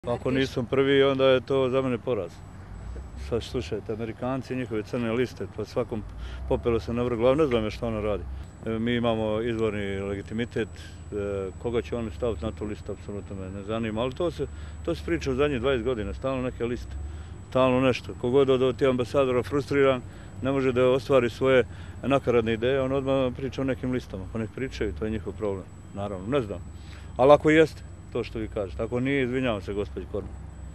Dodik: Neprimjereno miješanje SAD u unutrašnje stvari BiH
Predsjednik Republike Srpske Milorad Dodik ocijenio je da je apsolutno nedostojno jedne tako velike zemlje kao što su Sjedinjene Države, da se njihov ambasador otvoreno miješa u unutrašnje stvari bilo koje zemlje, pa i BiH.